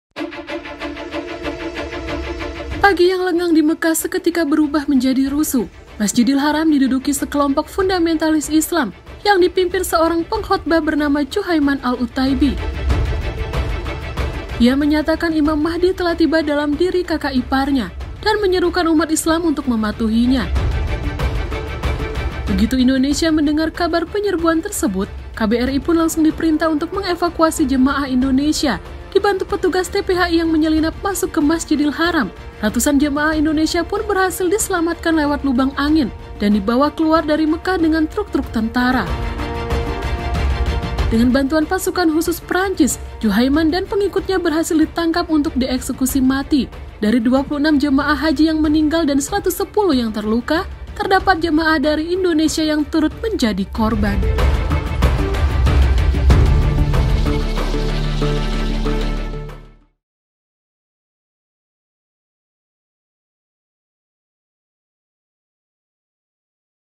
Kudeta Berdarah di Tanah Mekah | HISTORIA.ID
Sejumlah tentara elite Arab Saudi menduduki Masjidil Haram. Jemaah haji Indonesia jadi korbannya.